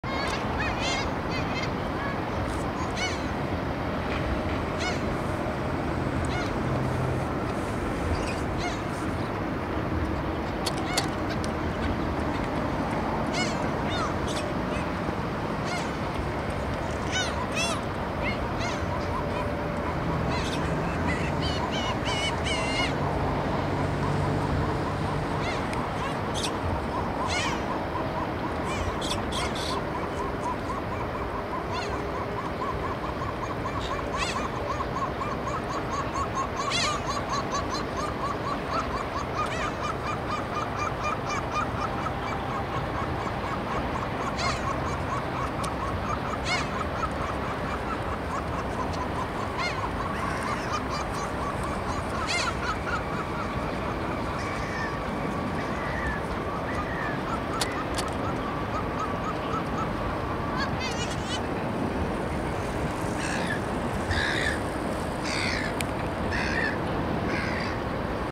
東京のウミネコ 愛の形 4月18日
2003年4月18日。
東京下町のウミネコたちの繁殖期の様子。
交尾の瞬間ばかり追いかけているわけではないのだけれど、目の前で見せつけるのでしょうがない。
さて、何処に「愛の巣」があるのかな？